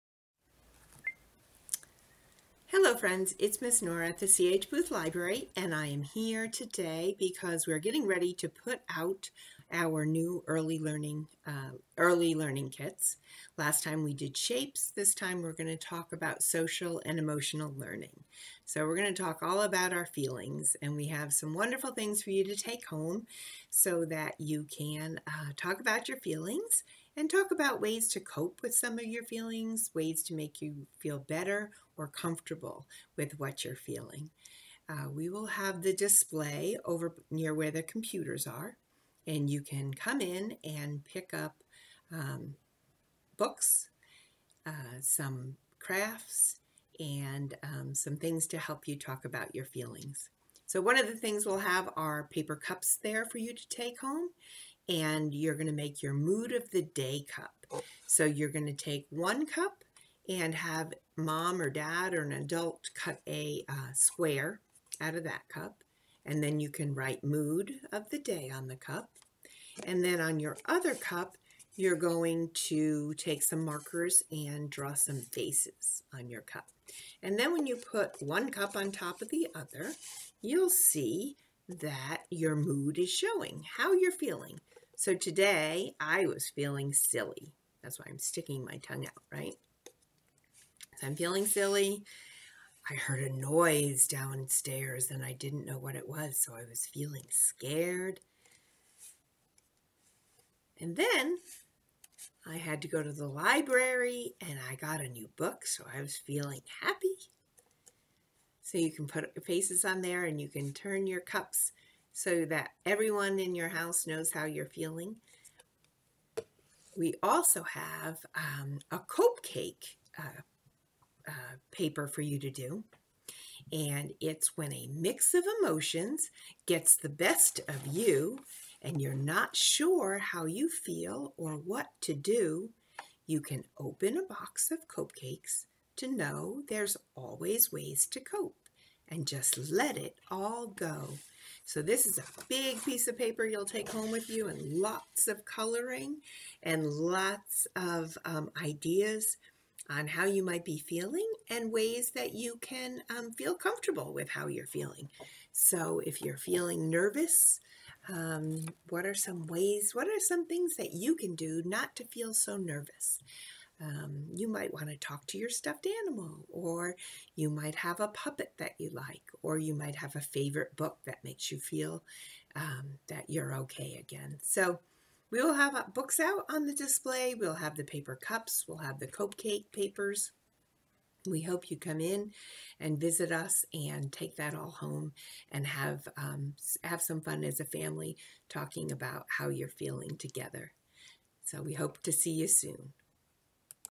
Social Emotional Learning: Mood Cups and Cope Cakes
Stop by the children's department to pick up the supplies to make "Mood Cups" and "Cope Cakes." Both activities are designed to get young children to identify and talk about their ever changing emotions. Parents, don't forget to bring home one of the suggested books to get the conversation started on the important topic of feelings and emotions.